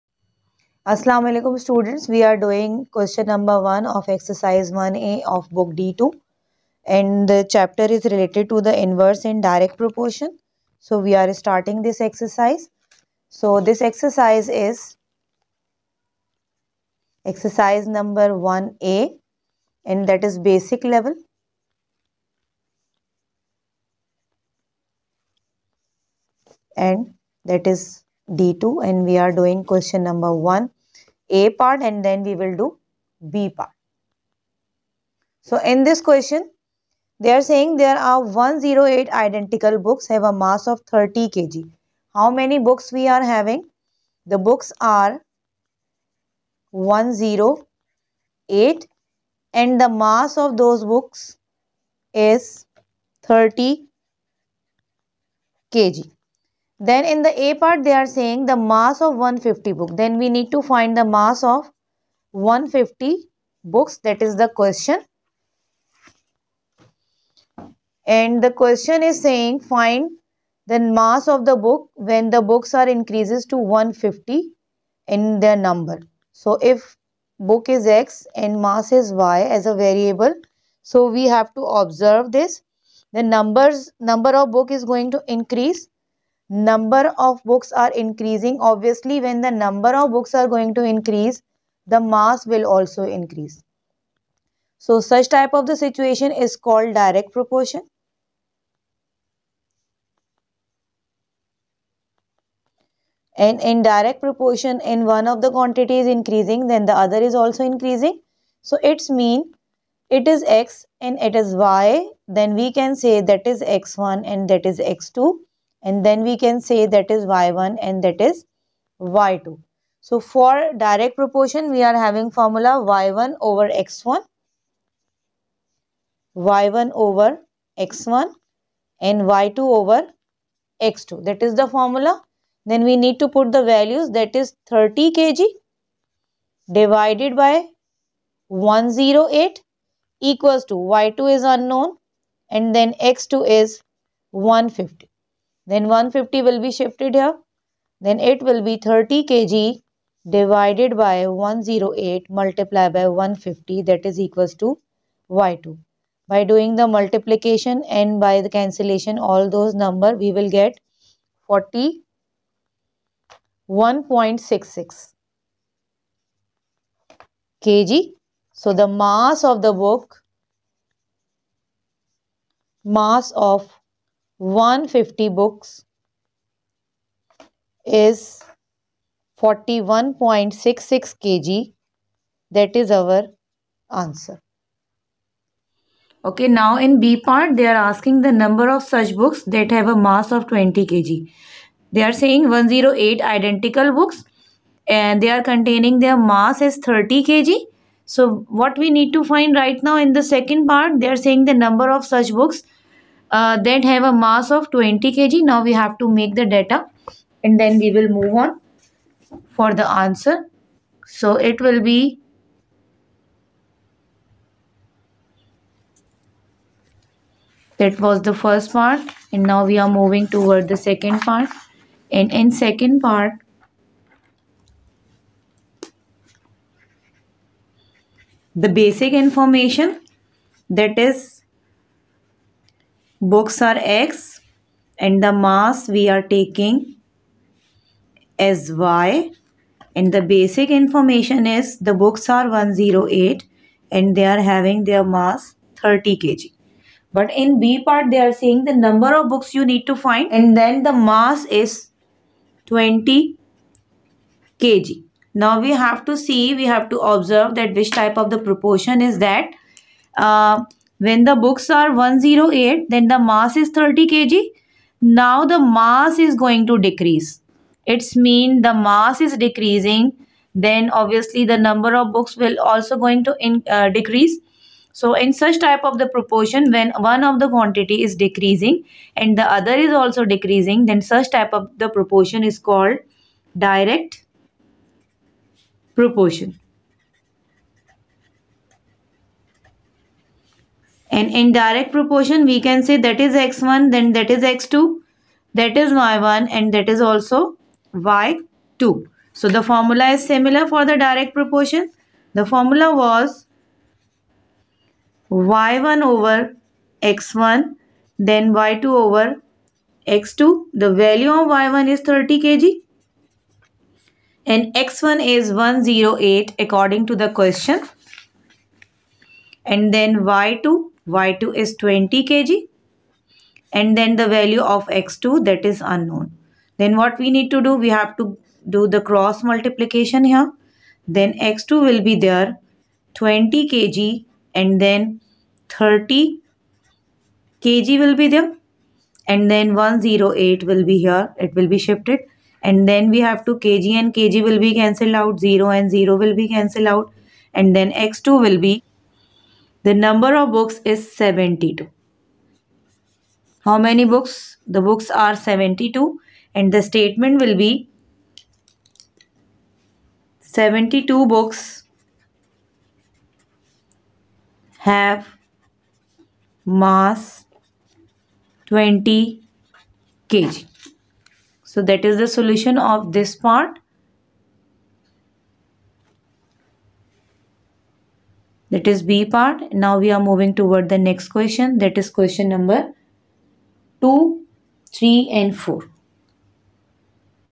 NSM:Ch#01(d-2) Direct and inverse proportion Ex#1.A Q#1(a,b)
In this video, we are connecting two variables to make linear equation if they are inversely proportional to each other and find the value of each variable in given condition.

new syllabus  mathematics 7th edition /elimination method /graphical metthod /class 8 maths/class 9maths/agha khan board/olevels maths/caie/inverse proportion/direct Proportion

l/0029VaDUHu18aKvIT9yJgH3w